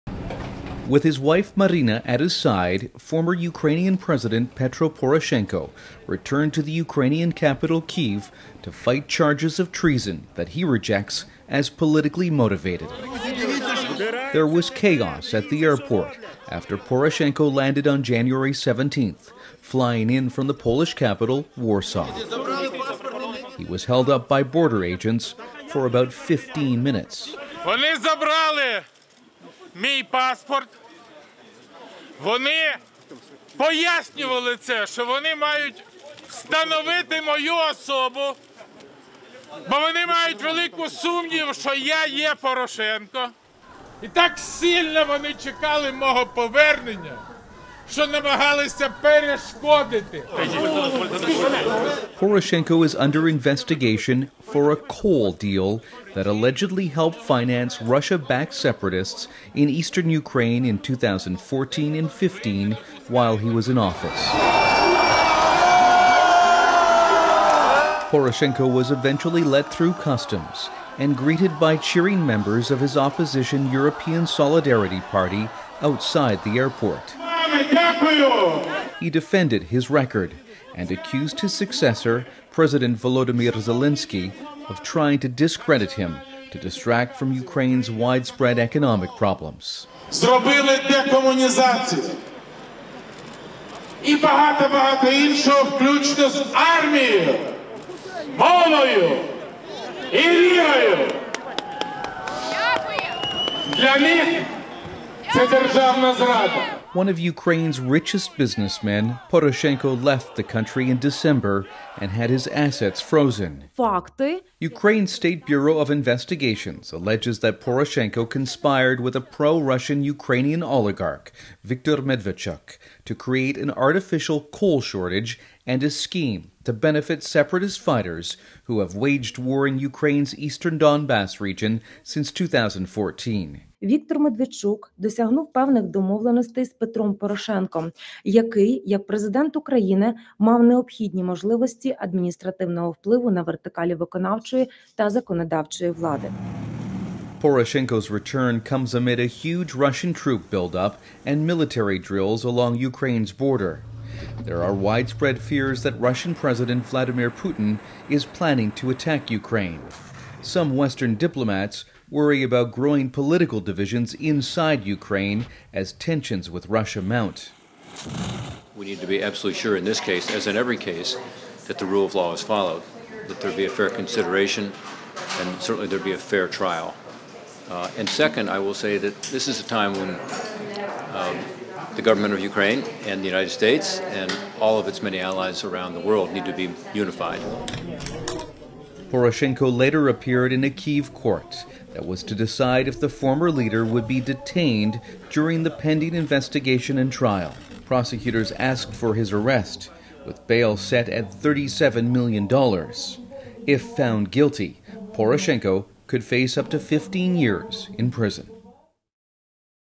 Defiant Ex-President Poroshenko Returns To Ukraine To Face Treason Charges
Former Ukrainian President Petro Poroshenko was defiant as he landed in Kyiv on January 17 to fight charges of treason that he rejects as politically motivated.